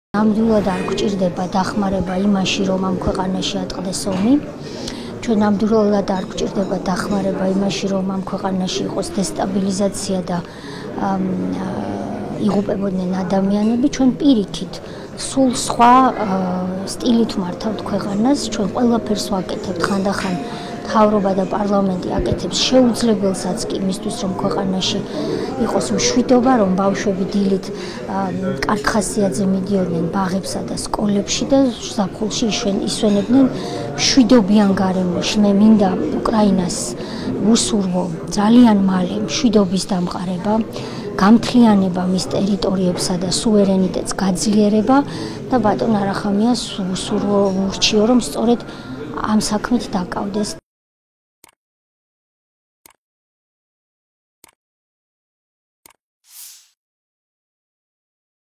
წულუკიანი: ჩვენ ყველაფერს ვაკეთებთ, ხანდახან შეუძლებელსაც, რათა ქვეყანაში იყოს მშვიდობა
▪️ „ჩვენ ყველაფერს ვაკეთებთ, ხანდახან შეუძლებელსაც, რათა ქვეყანაში იყოს მშვიდობა,“ — თეა წულუკიანი